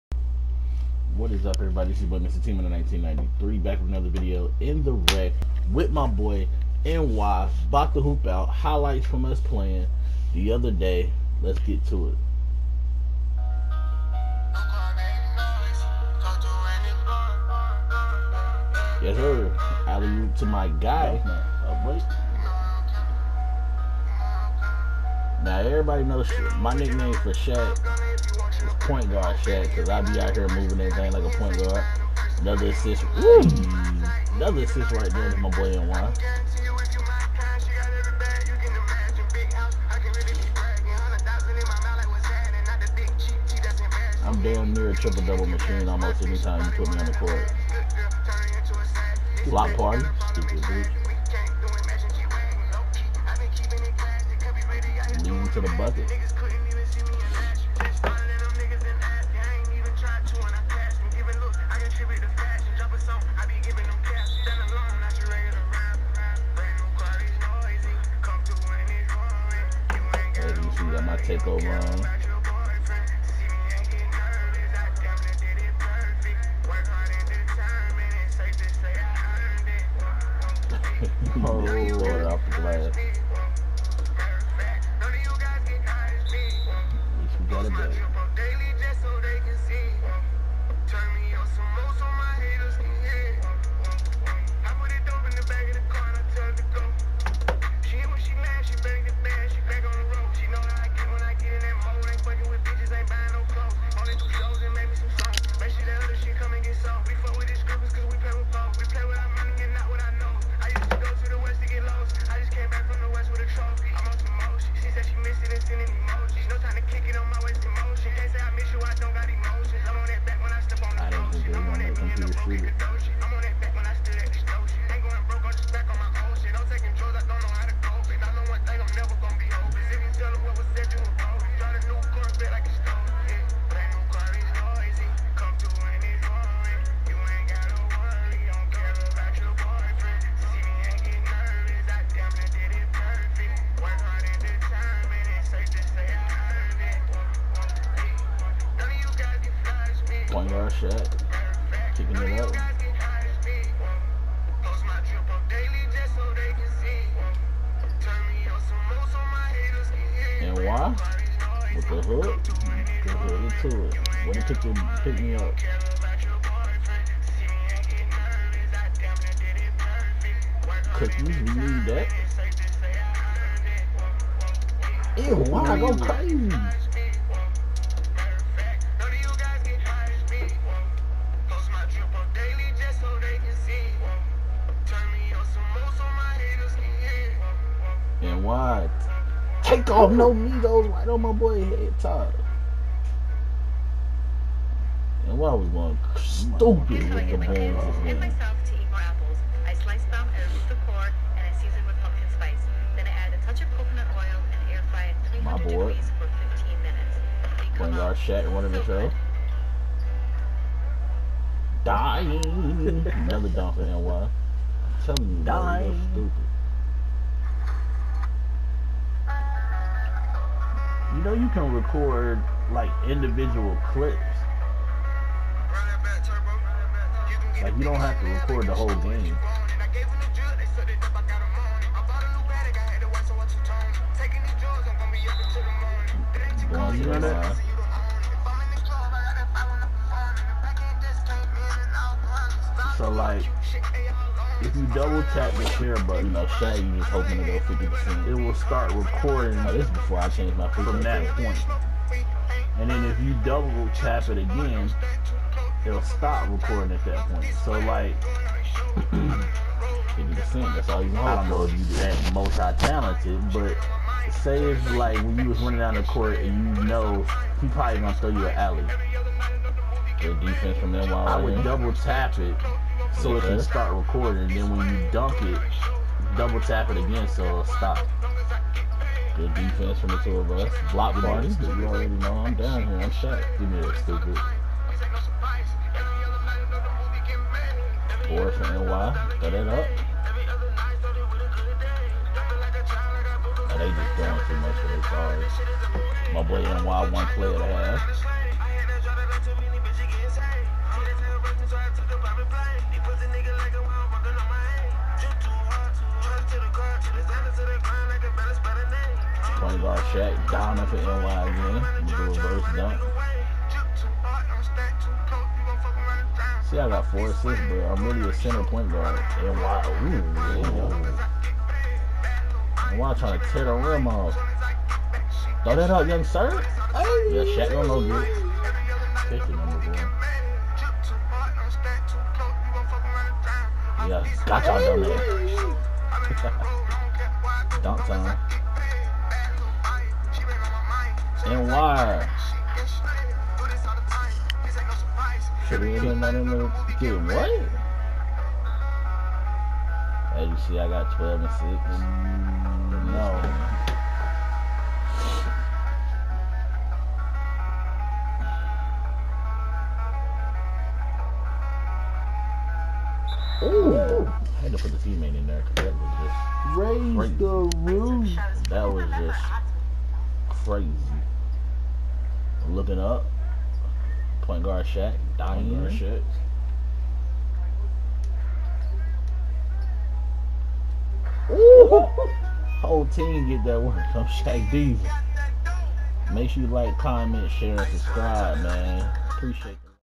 Center/Point Gawd Shaq in the Rec With My Boy NY
Center/Point Gawd Stepping on the Court Taking Over Shaq Diesel Style With My Boys!